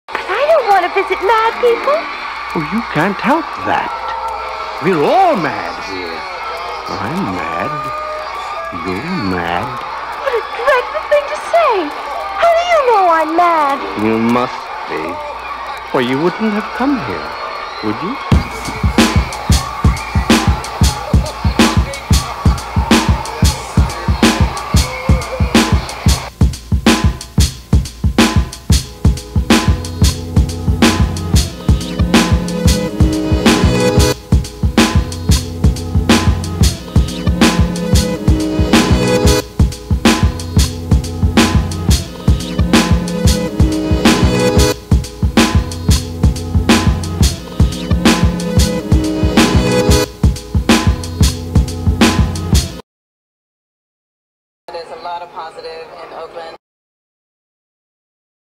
BlackOut Oakland 2012 1
Keep Downtown in The Black!
Blackout Oakland is our community alternative to the madness that is the day after Thanksgiving.
Come join us for Music, Food, Artists and Craftspeople and some of the finest and most fun and unique shopping in the Bay Area.
Support Local Owned Business